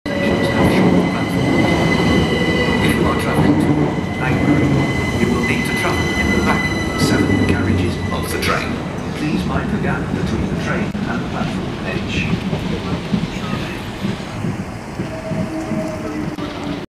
Gatwick Express Class 387 Electrostar train @Clapham Junction - 16th February 2019
A Class 387 Electrostar EMU (electric multiple unit), seen entering Clapham Junction railway station, while travelling along the Brighton Main Line towards Brighton.
This service was operated by Govia Thameslink Railway, under the Gatwick Express brand.